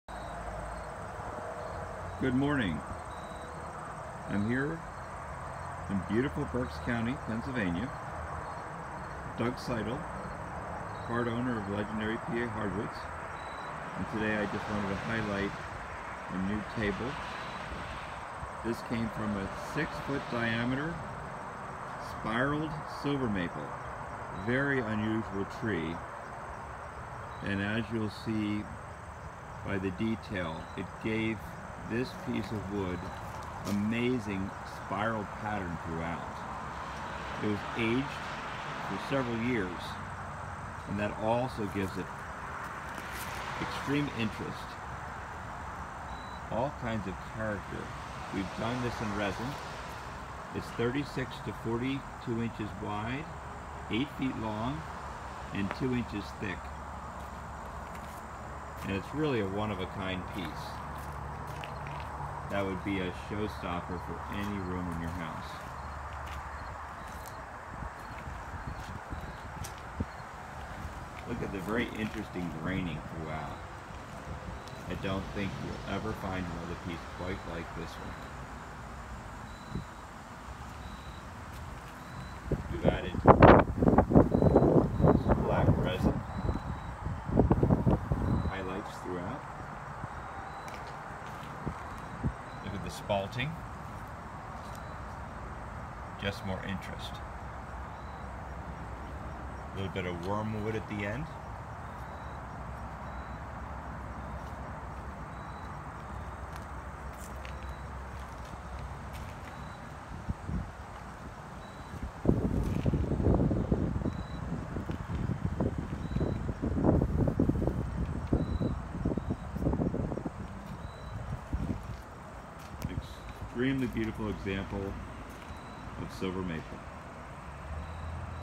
REALLY?!! Yup, this MAPLE is CRAZY.
Check out this beautiful Silver Maple Table top for sale. EMMA ROSE HARDWOODS. Live edge furniture, huge wood slabs. hundreds of professionally kiln dried wood. Huge.